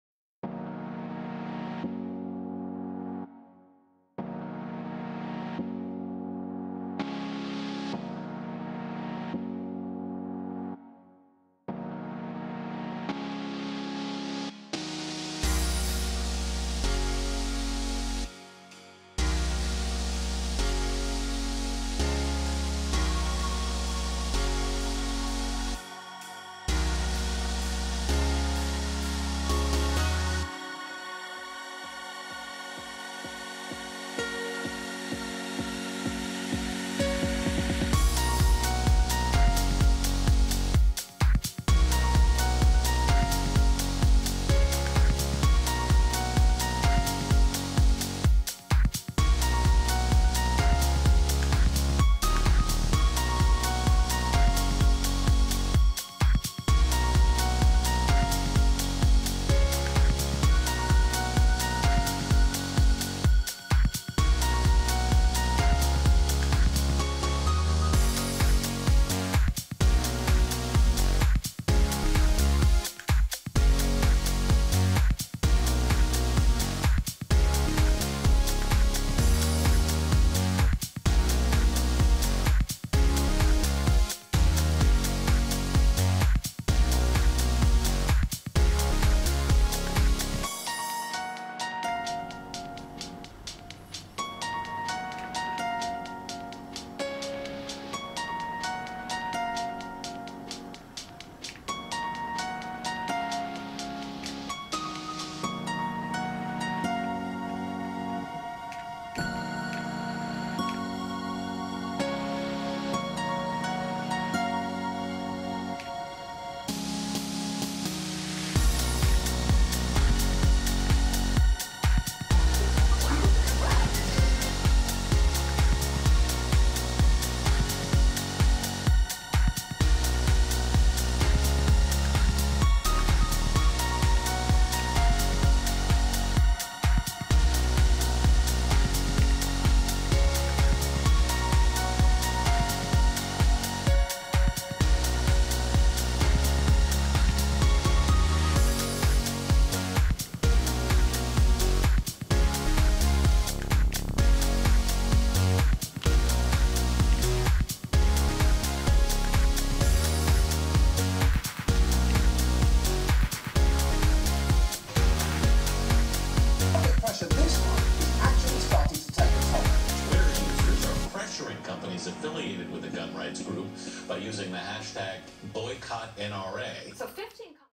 LG OLED55C7P 4K HDR Smart OLED TV Unboxing & Initial Setup
TechWalls presents unboxing and initial setup of the LG OLED55C7P 4K HDR Smart OLED TV.